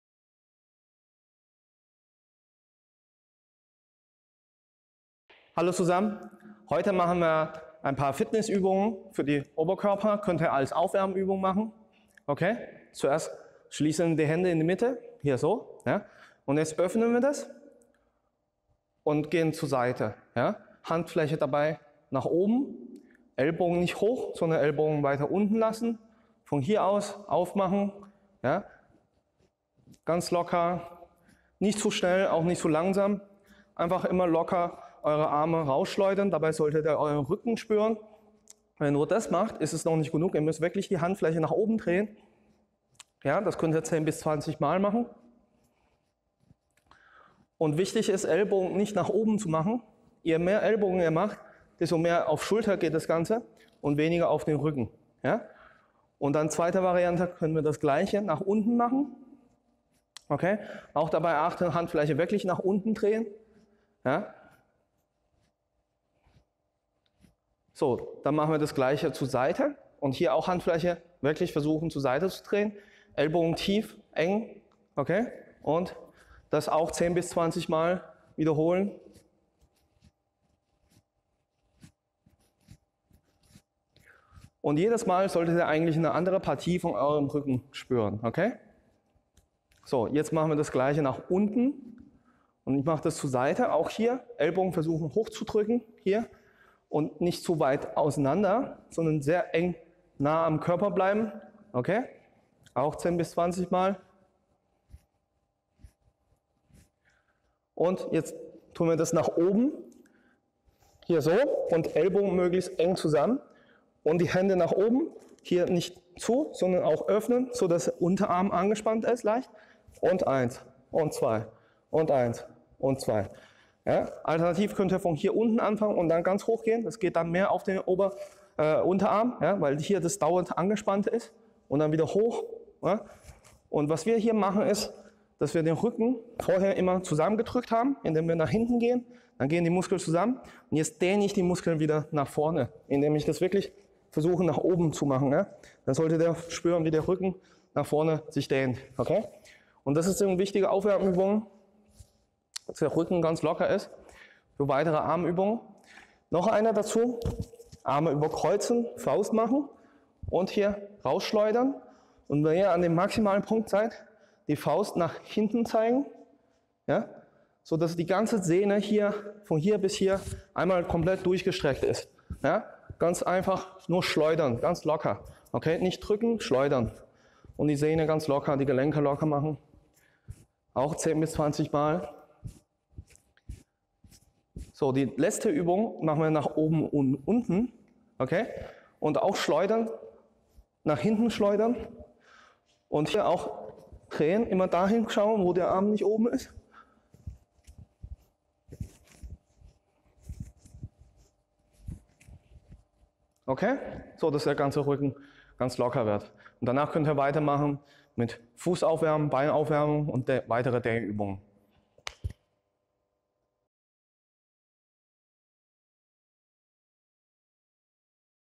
Kung Fu Fitness - 7 Übungen für den Rücken
Willkommen!

Wenn ihr zu lange falsch sitzt oder steht, bekommt ihr Rückenschmerzen und Nackenschmerzen. Dagegen können wir etwas tun.

In diesem Tutorial zeige ich euch 6 kleine Basis Qi Gong Übungen, die ihr entweder als Aufwärmen oder als Lockerung machen könnt.

Wichtig ist, Ellenbogen eng zum Körper zu halten, damit die Muskeln im Rücken beansprucht werden und nicht die Schulter.

Habt ihr Fragen? Einfach einen Kommentar hinterlassen.